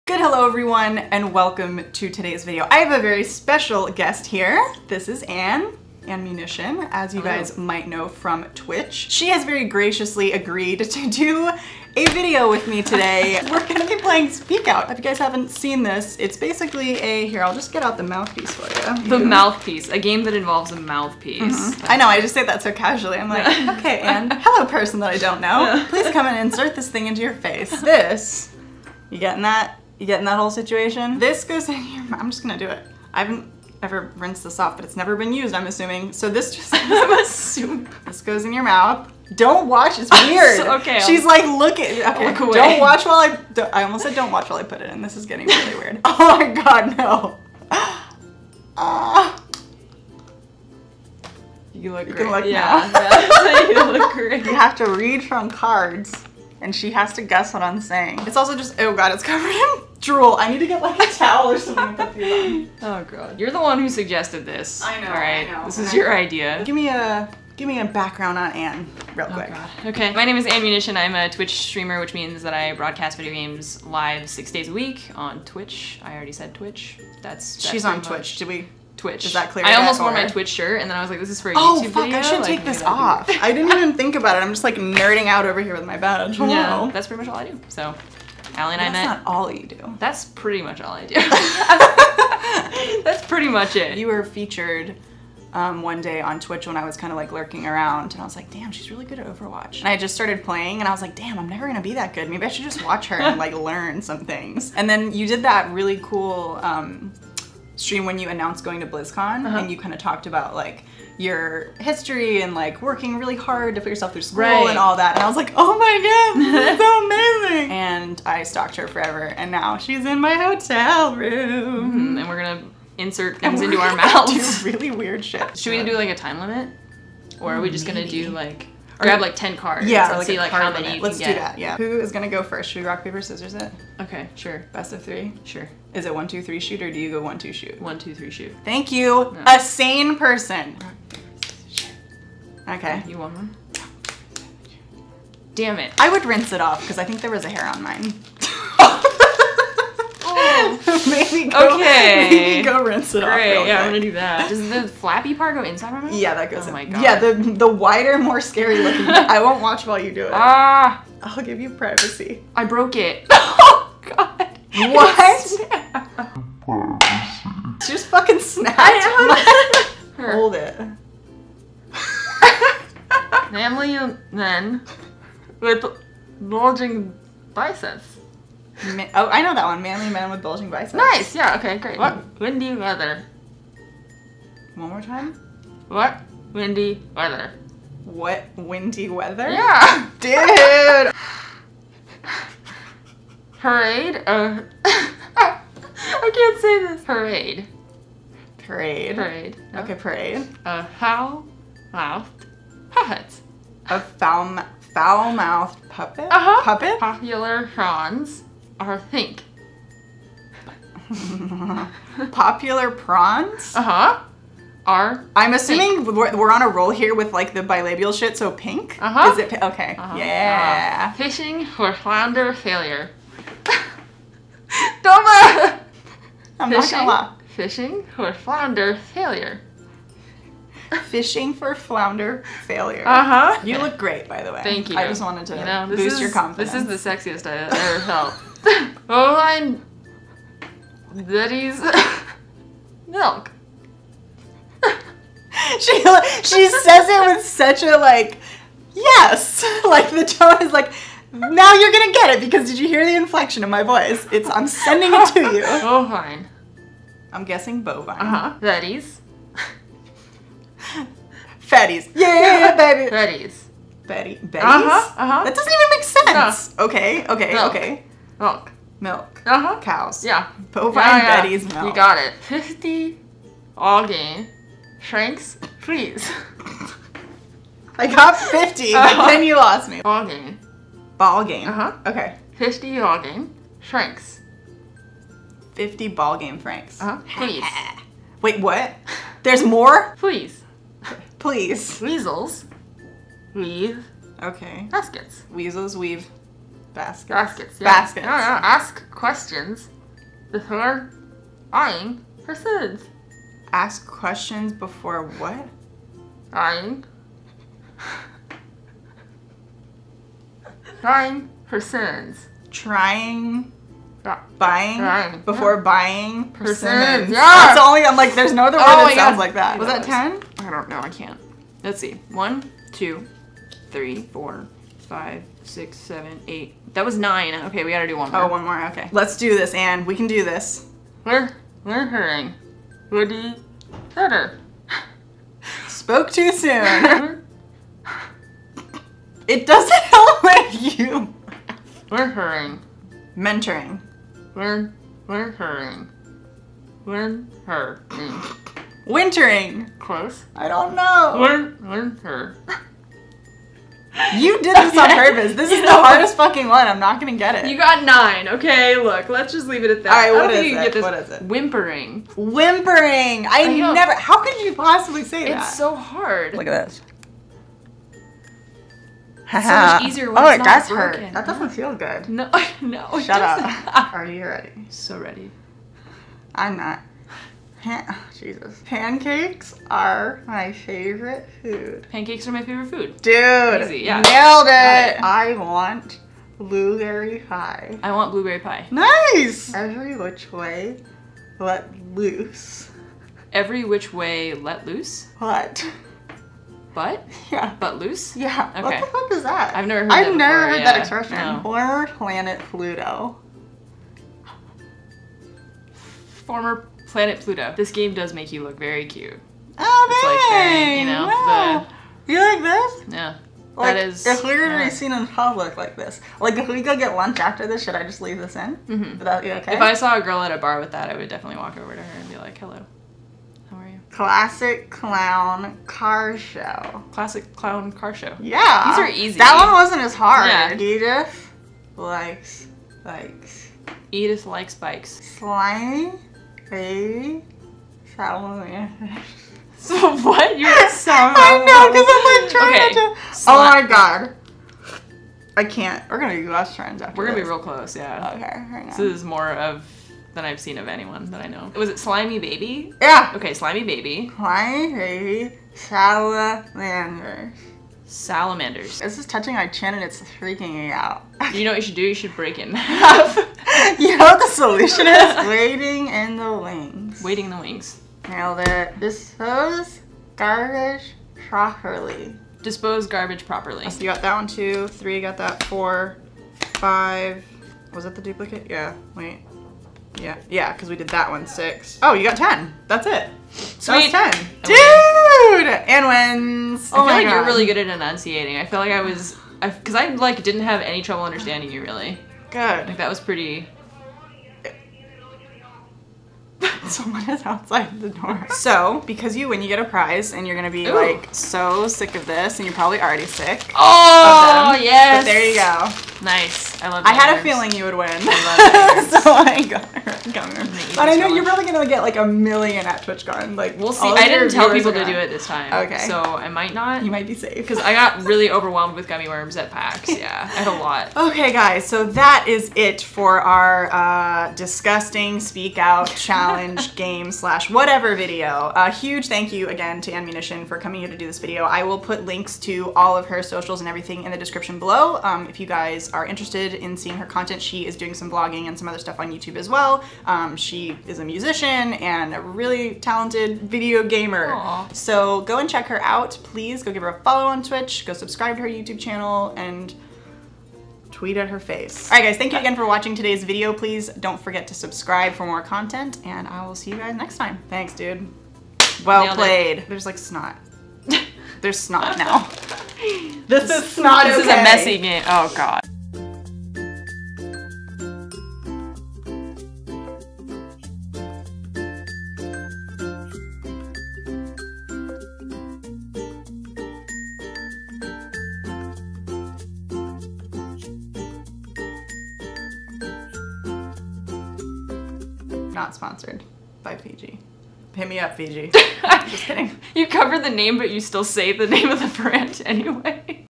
SPEAK OUT feat. Anne Munition
Today, you guys get to see a side of me you've never seen before. The inside... Of my mouth... This was the first time Anne and I met in person, and she was so brave and kind to agree to this ridiculous video! After editing and rewatching all of the clips, I realized that I miscounted. I only read her NINE cards! Guess that means we need a rematch? LOL

If you feel so inclined, send us something via ancient telegraphic delivery service:

allispeed